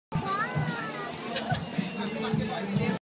Audrey on stilts for the 1st time!
Exchange your shoes for something more adventurous here at the inaugural Red Nose Walk where we walk a mile in someone's shoes! ;)